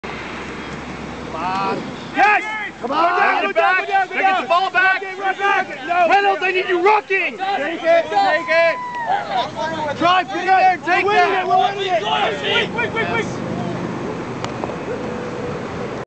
William and Mary Rugby Sept 2009
Penalty play v JMU,Brian Doyle at the mark